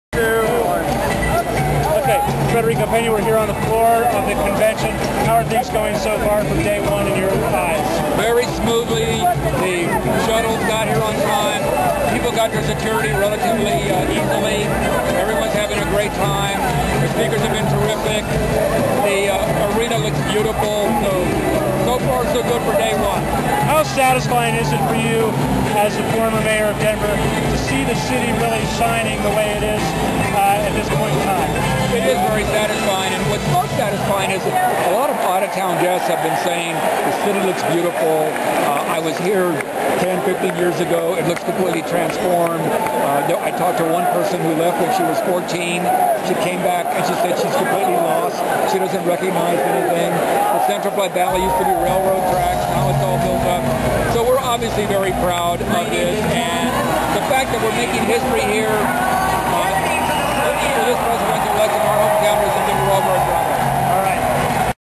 DNC Day 1 - Federico Pena
FOX 31's Charlie Brennan interviews former Denver mayor Federico Pena on the floor of the 2008 Democratic National Convention.